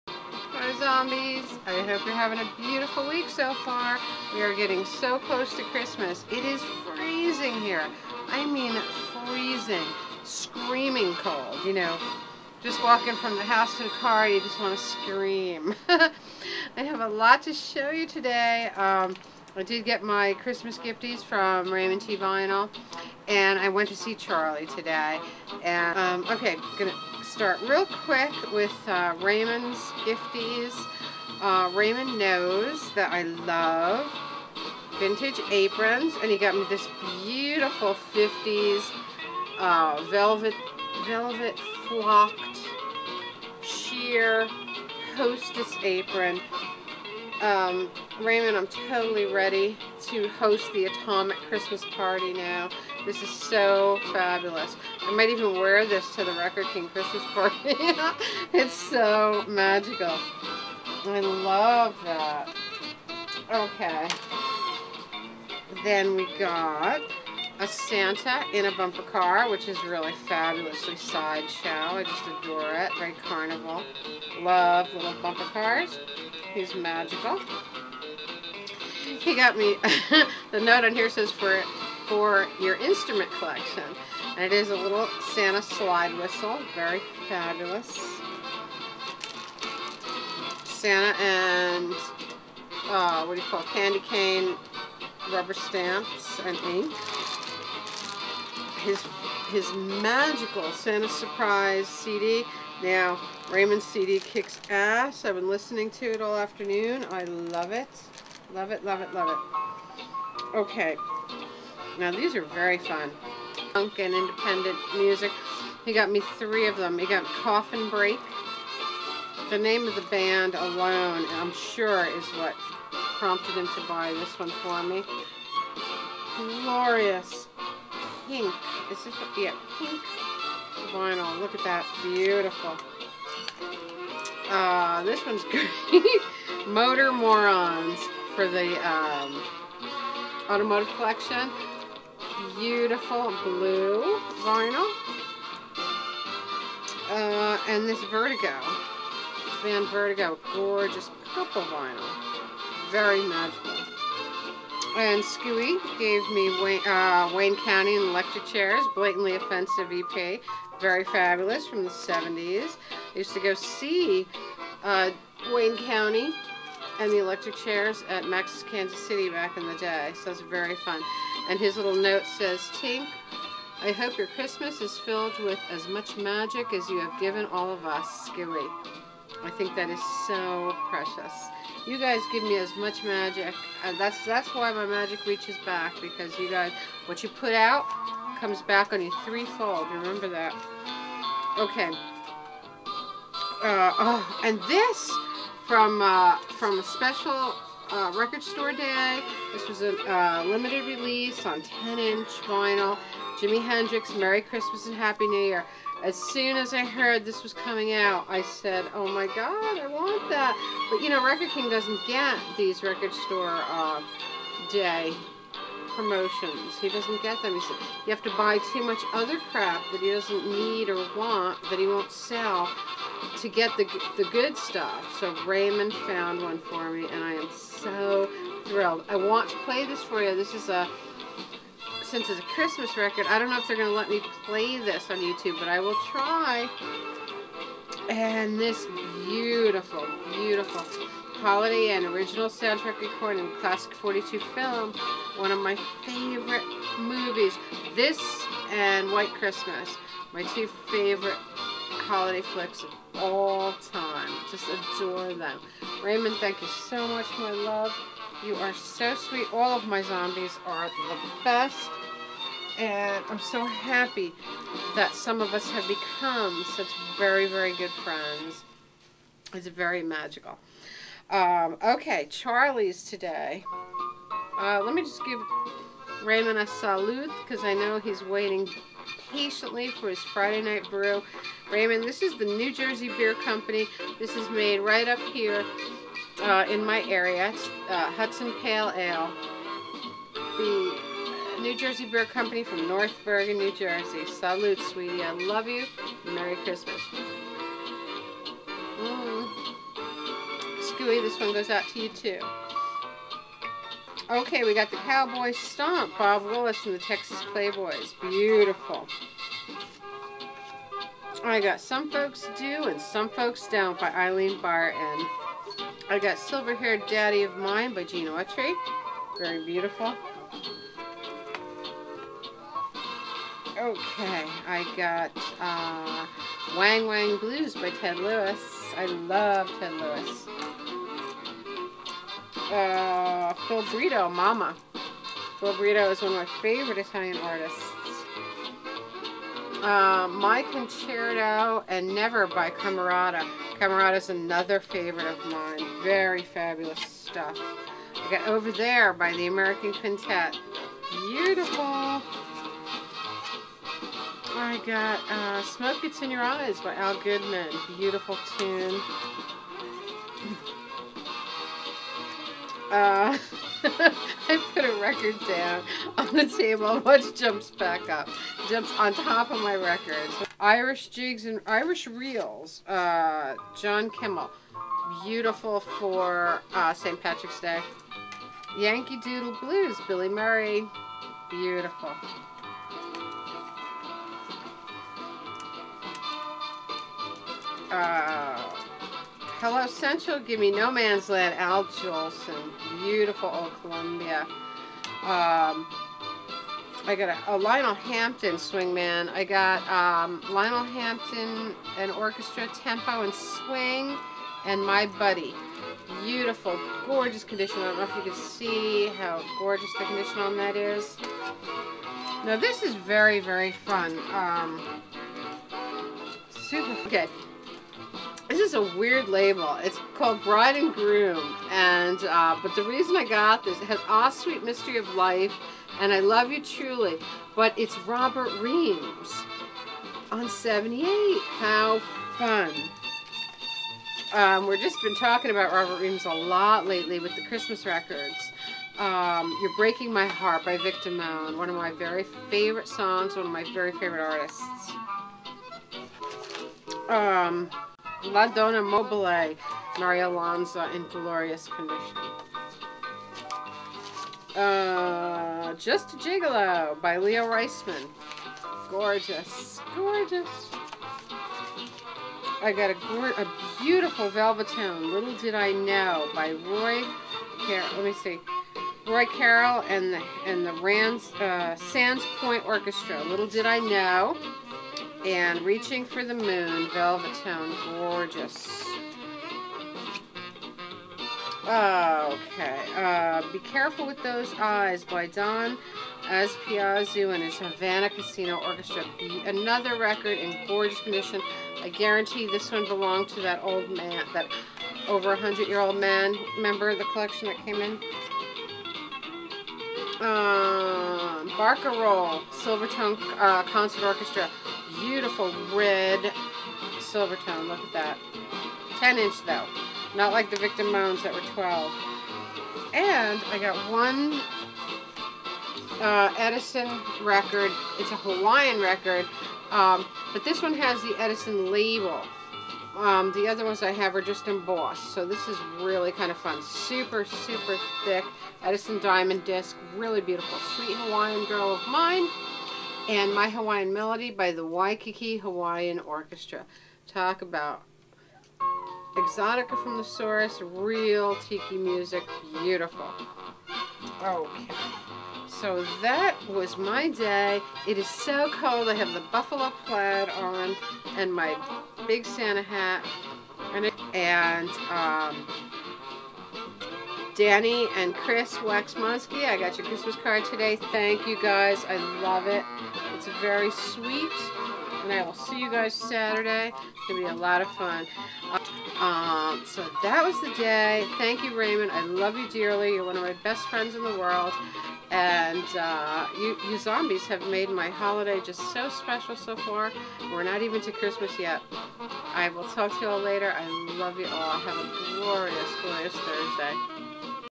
Tinkie and the RaymondT Gifties and Platter World
FABULOUS gifties from my dear Raymond along with finds from my beloved
Charlie. Hugs out for the Holidaze which are coming up FAST!!!